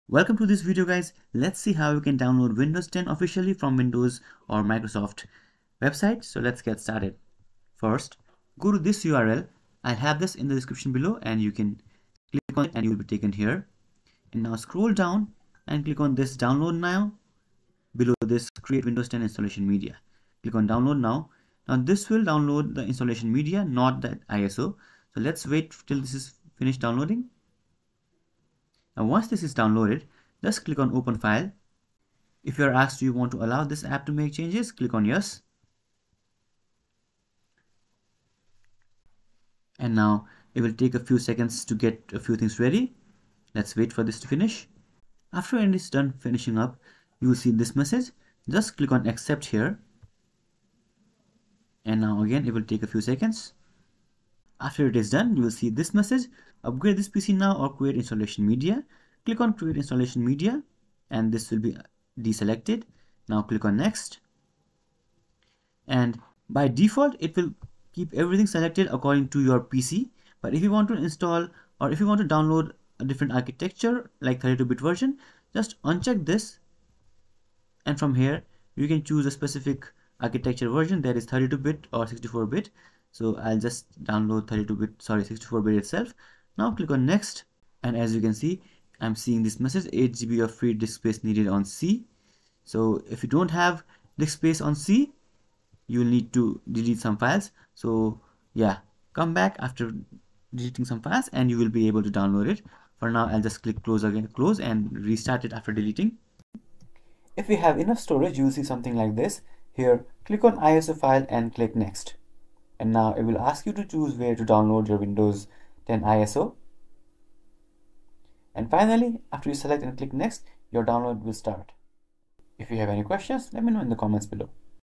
How to Download Windows 10 ISO from Microsoft (OFFICIAL 2024 METHOD)
In this video, I'll show you how you can download the windows 10 iso from official Microsoft website.
After downloading the windows 10 iso from microsoft site, you can use it to create a bootable USB disk or install it in virtual box.

1. Go to the link above
2. Click download media to download installation media
3. Open installation media
4. Choose ISO in the setup wizard
5. Windows 10 ISO will be downloaded

Note: Make sure you have 8GB or more space available in your C drive to download windows 10 ISO file.
By the end of the video, you'll see how you can download windows 10 ISO file.